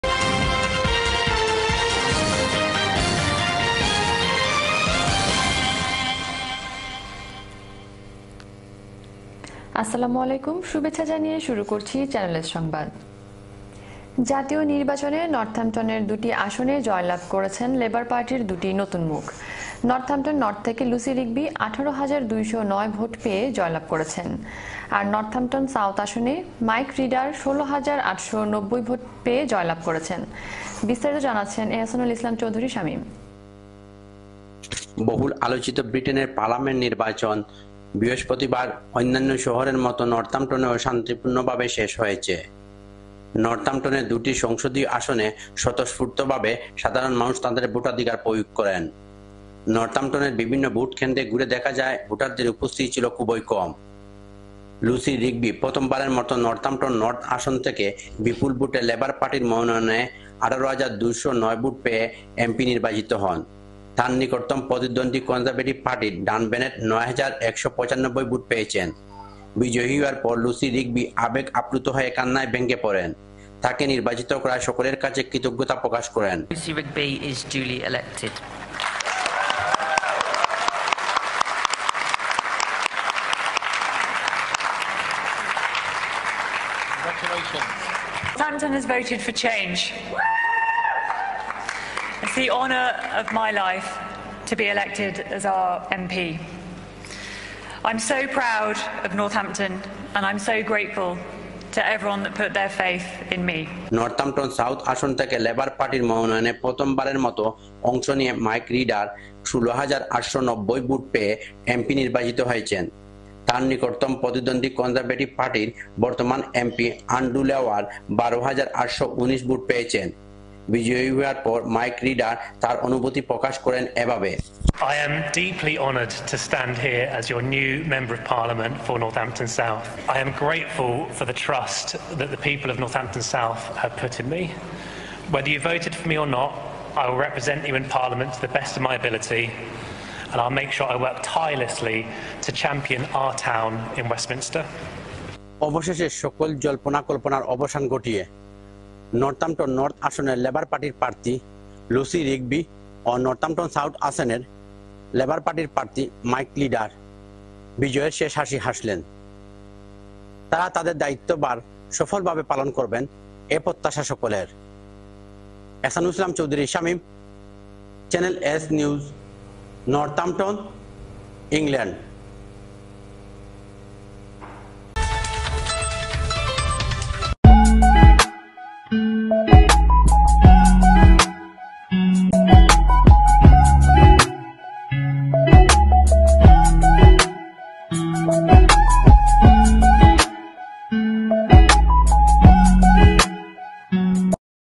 Uk Parliament Election 2024.Northampton North & South Results।।Channel S News।।
ব্রিটেনের নর্থাম্পটনের দুটি আসনে পারলামেন্ট  নির্বাচনে লেবার পার্টির নতুন মুখ দুই জন নির্বাচিত হয়েছেন।তারা হলেন নর্থাম্পটন নর্থ থেকে লুসি রিগবি ১৮হাজার ২ শ ৯ ভোটে পেয়ে জয় লাভ করেছেন।আর নর্থাম্পটন সাউথ আসনে মাইক রিডার ১৬ হাজার ৮ শ ৯০ ভোট পেয়ে জয়লাভ করেছেন।বিস্তারিত জানাচ্ছেন এহসানুল ইসলাম চৌধুরী শামীম